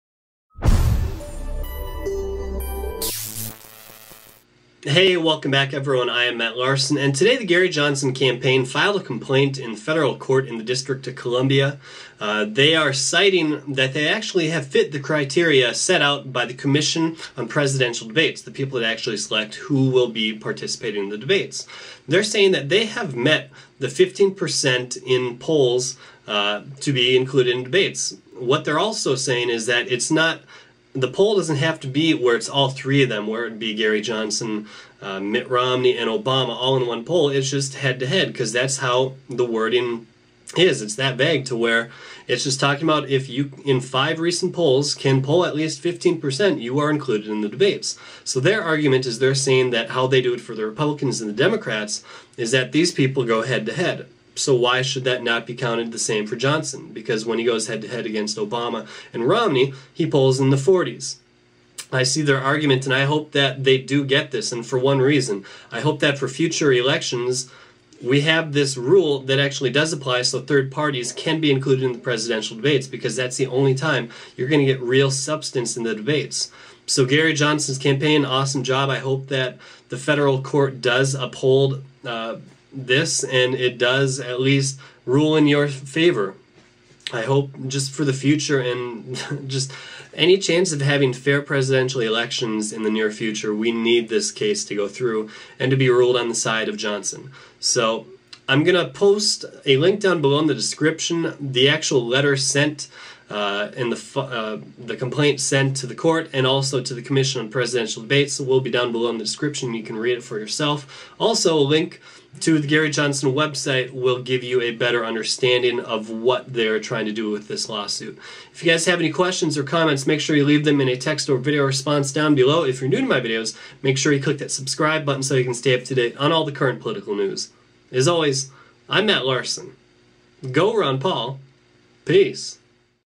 Gary Johnson Files Complaint In Federal Court Against Commission On Presidential Debates
Johnson has asked the D.C. federal court to intervene in the presidential debates on account of polling fallacy.